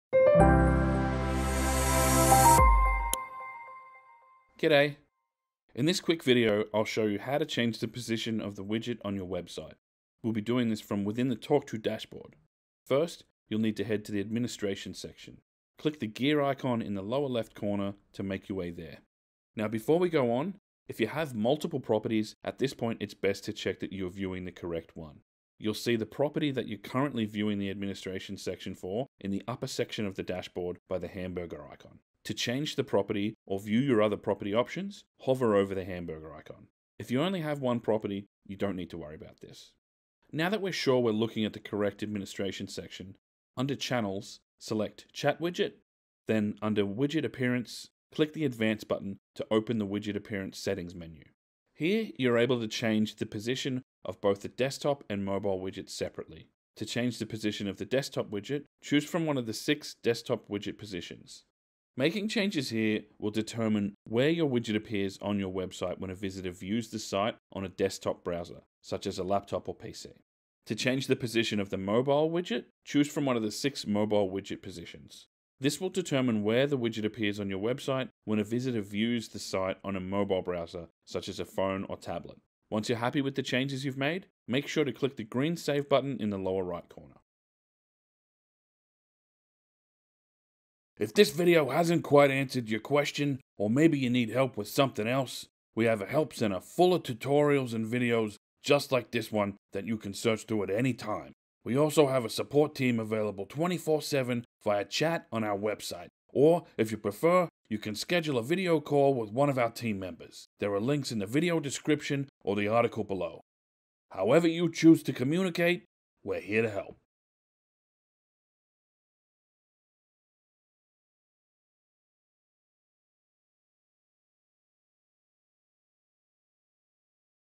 Changing the widget position
Follow the steps in this video to change the position of the widget on your website.